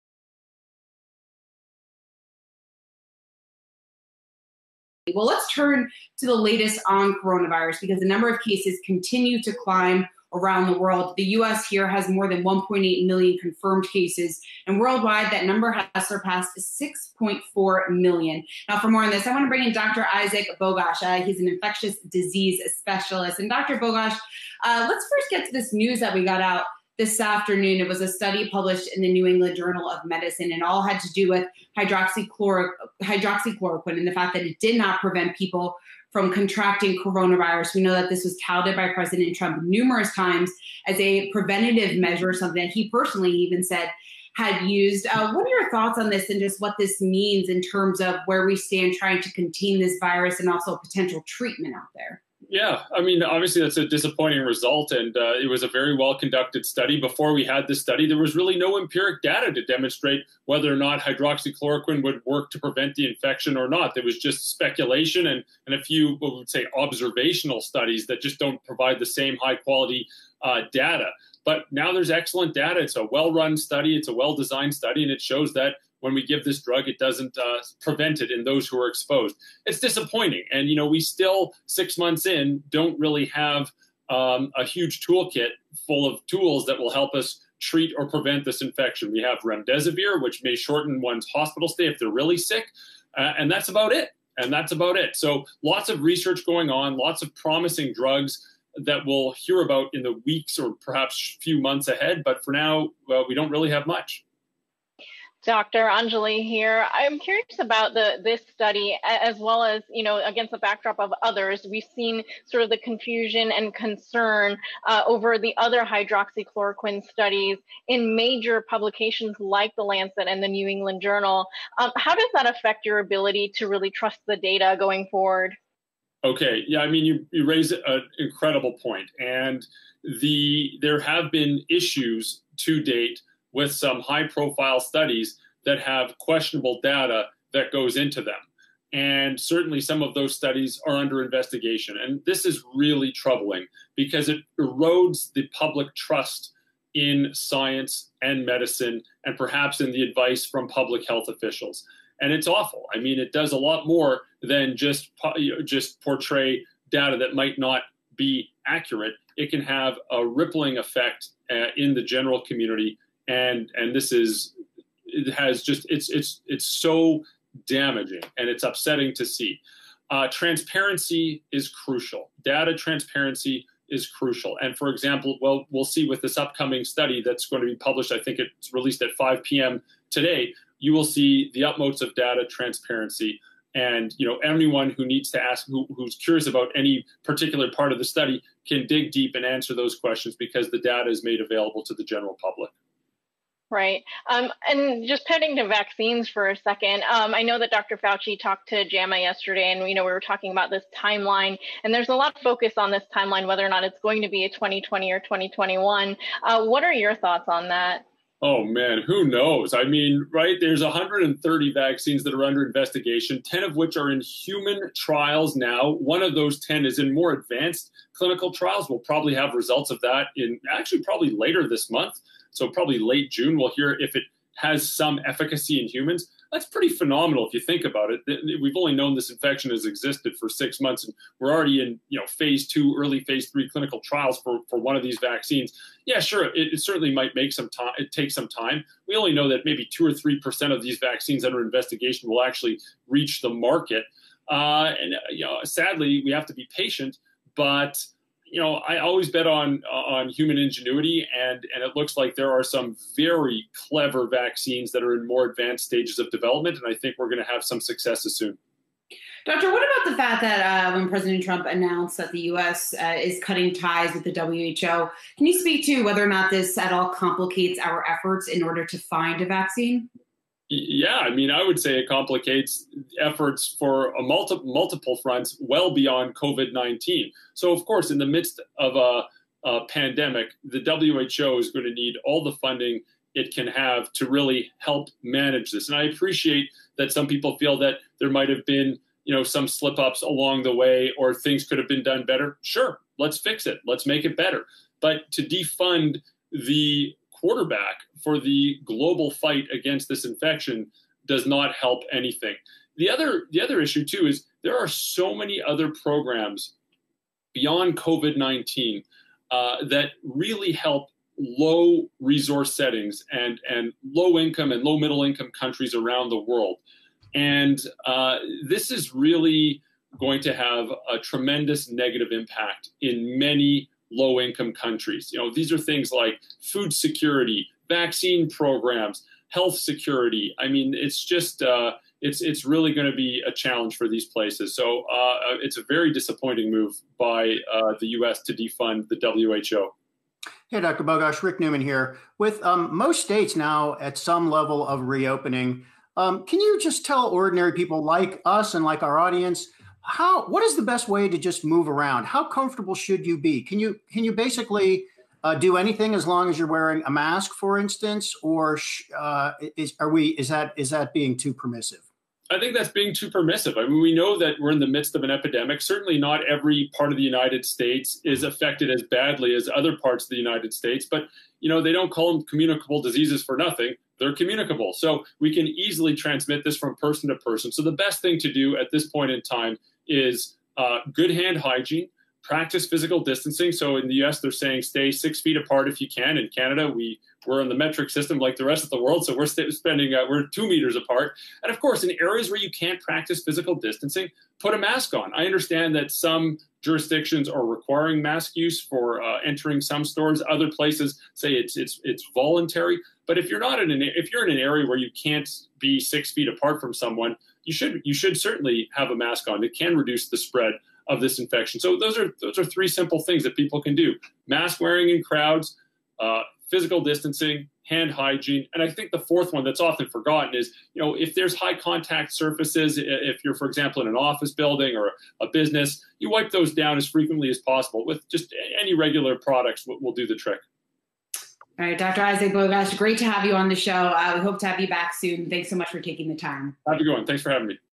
Breaking down the latest news on coronavirus vaccine and treatment
A new study from the New England Journal of Medicine was released on Wednesday, highlighting the issues with hydroxychloroquine and its effectiveness. Infectious Diseases Specialist Dr. Isaac Bogoch joins The Final Round to break down the latest on the coronavirus and the race for a vaccine.